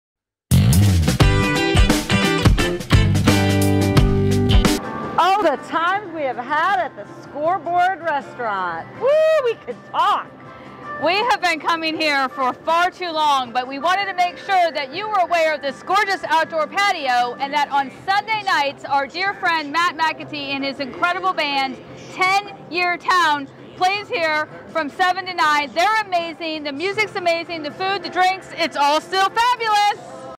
Check out the Scoreboard Bar and Grill in Nashville!
Langer Loves Local and Langer LOVES Scoreboard Bar Nashville ❤️ We've had some great times there and love listening to Ten Year Town on Sunday nights! So head on out there to have a great meal, drink and listen to some music! 🎼